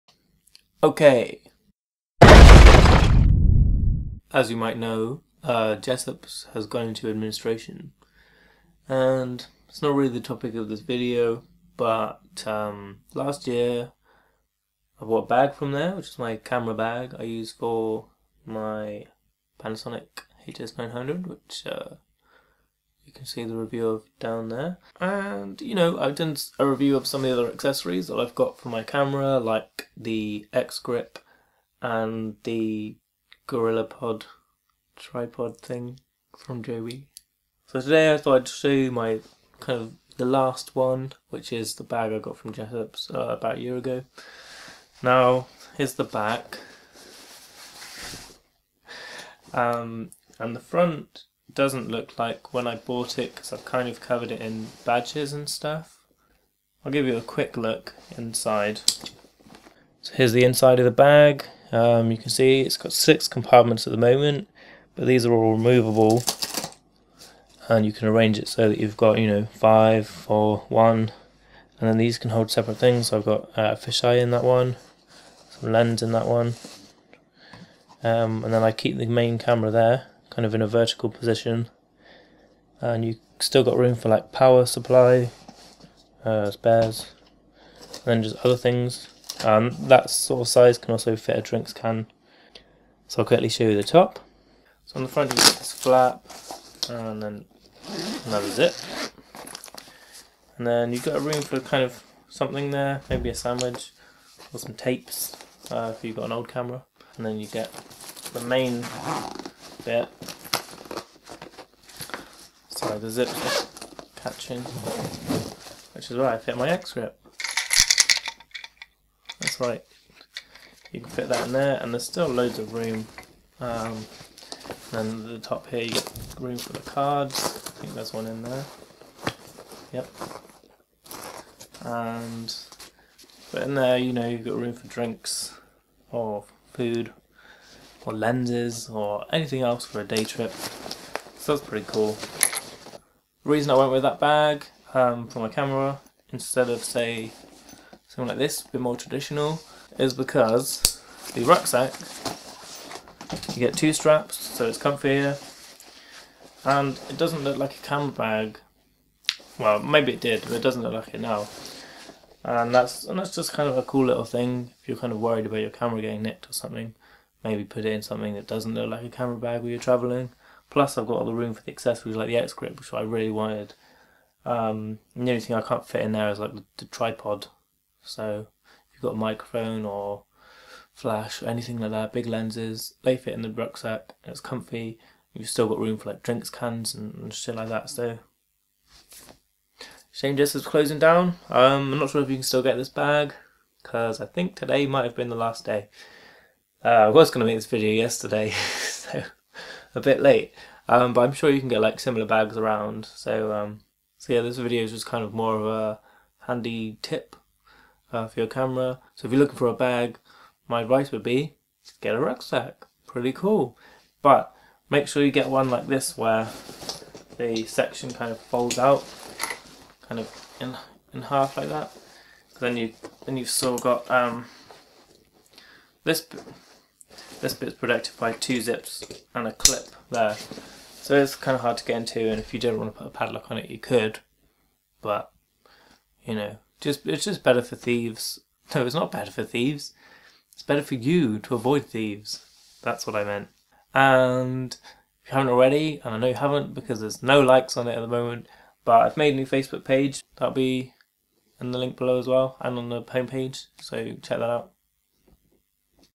Jessops Tecno Rucksack | Review + News
So Jessops has closed now... better late than never here is the review of their Tecno Rucksack. I think you can still buy this in other online stores.

*Re-uploaded with clean audio